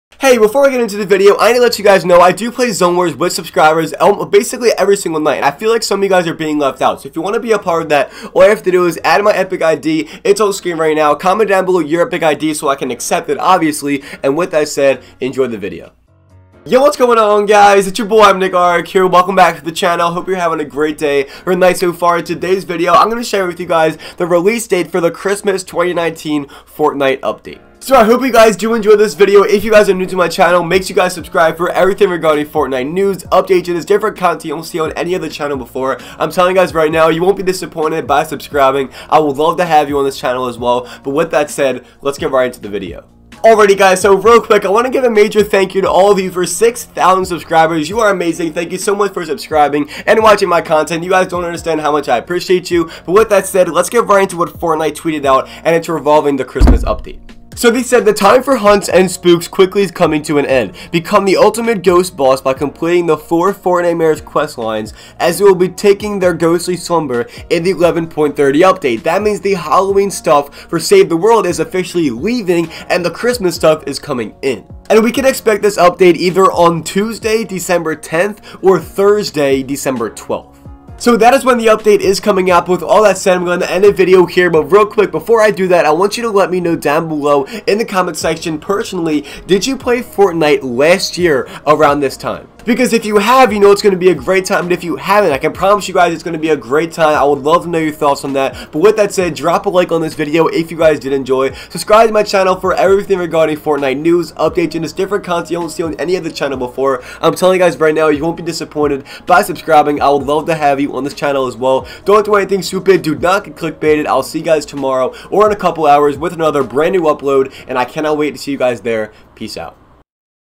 *NEW* Christmas Update RELEASE DATE! 2019 | Fortnite
Hello! This is *NEW* Christmas Update RELEASE DATE! 2019 | Fortnite

In this video, I showed you guys the release date for the Christmas 2019 update in Fortnite! This Christmas patch update can consist of new skins, pickaxe, gliders, emotes, wraps & MUCH MORE.. I Hope You Guys Enjoyed This Video!!

Im NickArg